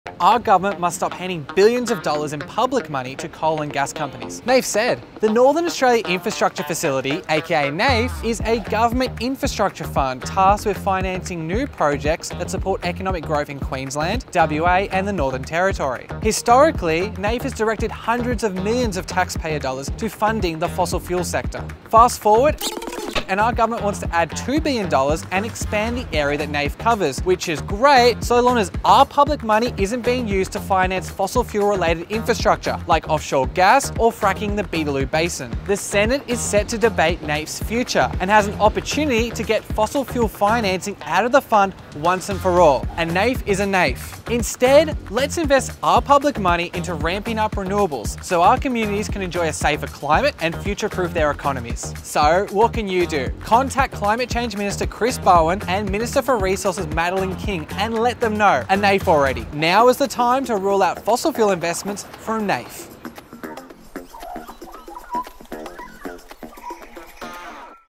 $7 billion in public money for gas?
Next week, our Parliament will vote on increasing the funding of the Northern Australia Infrastructure Facility (NAIF) to $7 billion. But the minister responsible for NAIF is Resources Minister Madeleine King, who is on the record as a supporter of the polluting gas industry.

We must act now to remove any opportunity for this public money supporting projects like Woodside's Scarborough Gas Project and fracking the Beetaloo Basin.

E-NAIF already! Let's all speak up to create a surge of public pressure urging the Climate and Resources Ministers to rule out $7 billion NAIF from funding fossil fuels!